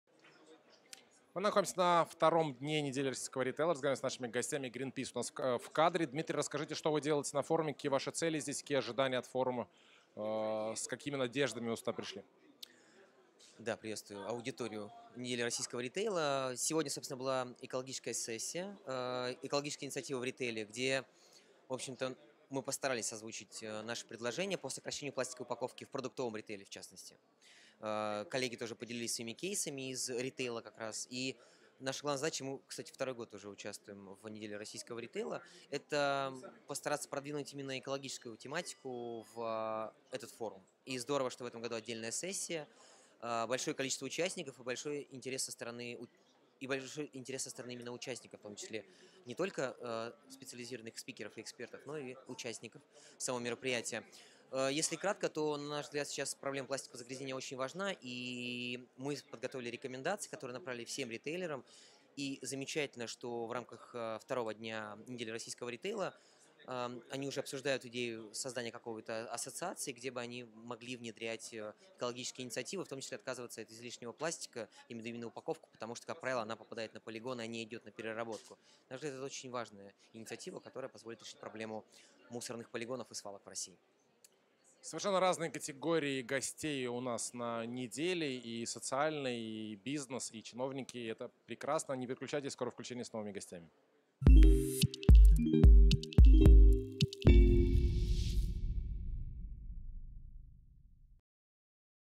Дмитрий Нестеров, Greenpeace России
Дмитрий Нестеров, Greenpeace России, рассказал о сессии «Экологические инициативы ритейла»

Детали интервью в видео: